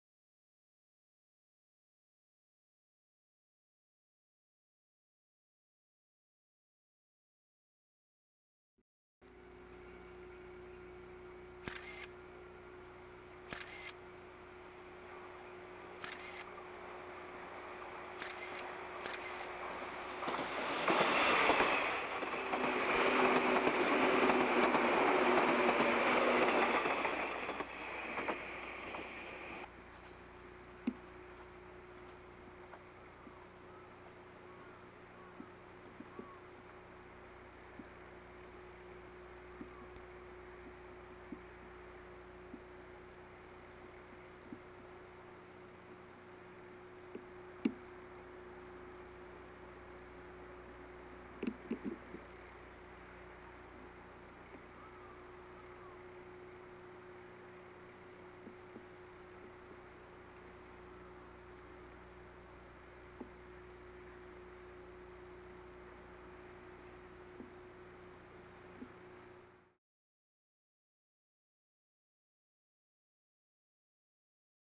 中央東線 すずらんの里駅 パノラマエクスプレスアルプス号通過
1987年11月撮影　中央東線　すずらんの里駅を通過するパノラマエクスプレスアルプス号です。中央東線　165系